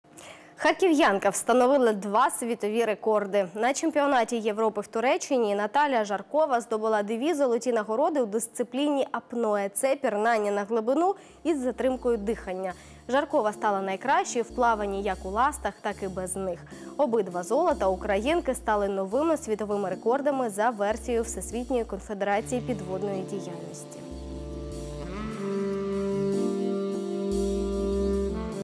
Харків’янка встановила два світові рекорди
На чемпіонаті Європи в Туреччині Наталя Жаркова здобула дві золоті нагороди у дисципліні – апное. Це пірнання на глибину із затримкою дихання. Жаркова стала найкращою в плаванні як у ластах, так і без них. Обидва золота українки стали новими світовими рекордами за версією Всесвітньої конфедерації підводної діяльності.